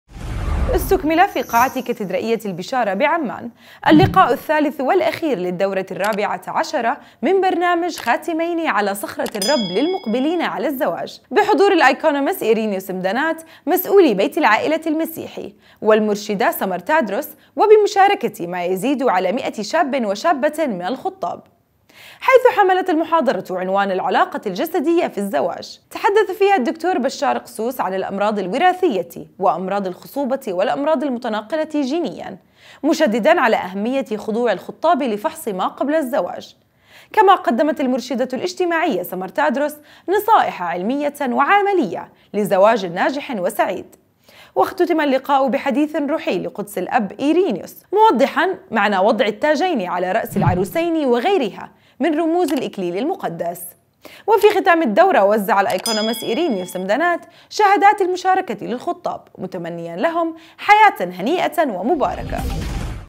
استكمالُ الدورةِ الرابعة عشره من برنامجِ "خاتِمَينَ على صخرةِ الرَّبّ
استُكمِلَ في قاعةِ كاتدرائيّةِ البشاره بعمان الّلقاءُ الثّالثُ والأخير للدّورةِ الرابعةِ عشره من برنامجِ خاتِمينَ على صخرةِ الرَّبّ للمُقبلينَ على الزواجِ / بحضورِ الإيكونوموس إيرينيوس مدانات مسؤولِ بيتِ العائلةِ المسيحيّ والمرشده سمر تادرس وبمشاركةِ ما يزيدُ على مئةِ شابٍ وشابةٍ من الخُطّاب / حيثُ حمَلت المحاضرةُ عنوان العلاقةُ الجسديةُ في الزواج / تحدّثَ فيها الدكتور بشار قسوس عن الأمراضِ الوراثيةِ وأمراضِ الخصوبةِ والأمراضِ المُتَناقَلَةِ جينياً / مشدداً على أهمّيةِ خضوعِ الخطّابِ لفحصِ ما قبلَ الزّواج / كما قدمت المُرشدةُ الاجتماعيه سمر تادرس نصائحَ عمليةً وعلميةً لزواجٍ ناجحٍ وسعيد / واختُتِمَ الّلقاءُ بحديثٍ روحيّ لقدسِ الأبِ إيرينيوس موضحاً معنى وضعِ التّاجَينِ على رأسِ العروسَين وغيرِها من رموزِ الإكليلِ المقدّس / وفي ختام الدوره وزّع الإيكونوموس إيرينيوس مدانات شهاداتِ المشاركةِ للخُطّابِ متمنياً لهم حياةً هنيئةً ومباركه